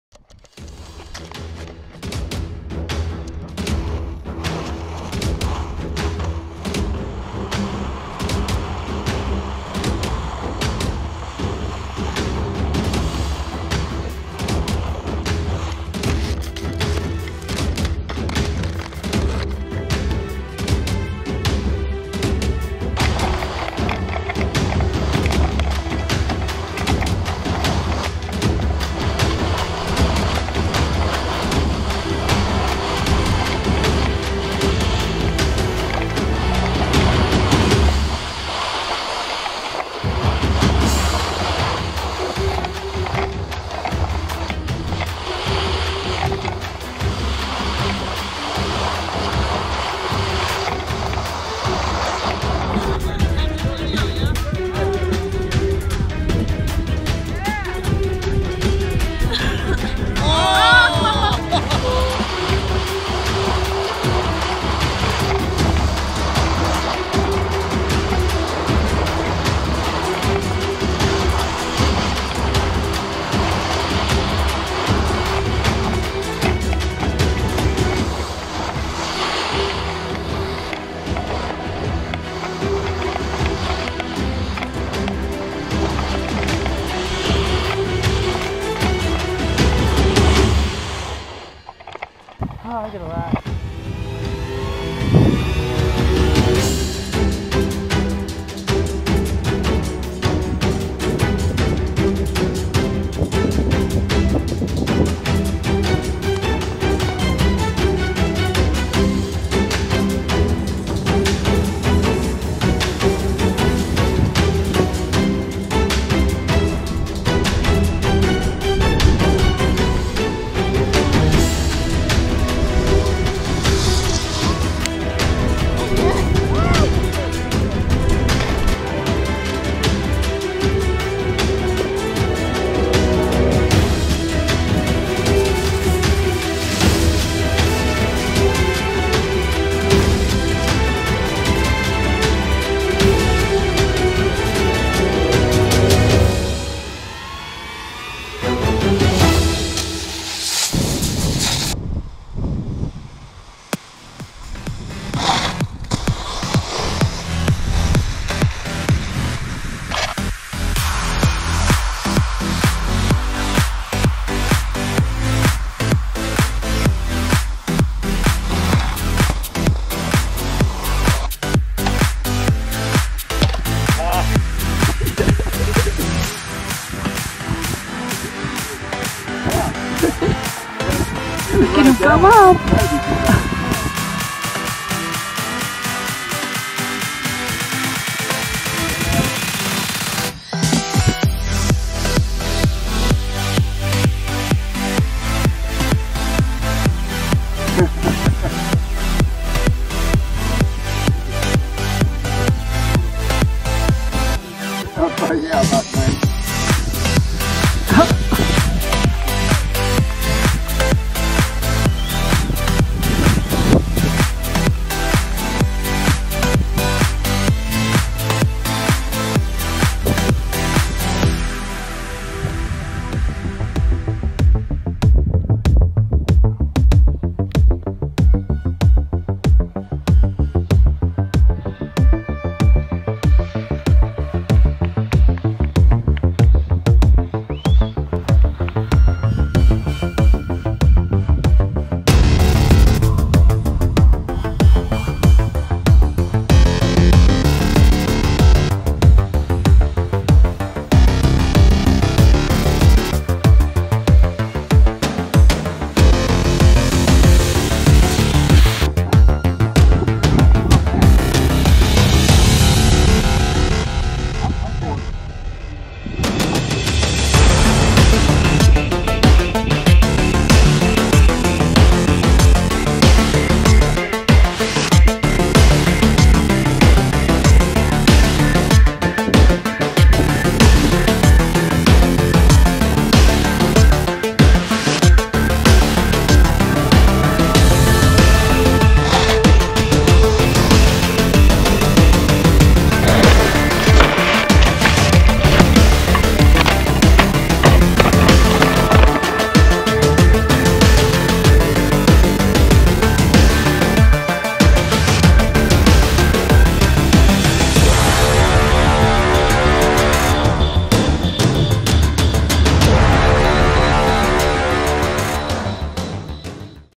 Riding Rocks Episode 2
Cinder Boarding the Cinder Hills of Flagstaff's old volcano slopes. Progression is in session and I can't wait to see how far we can go. Nico Loco Music productions bringing you episode 2.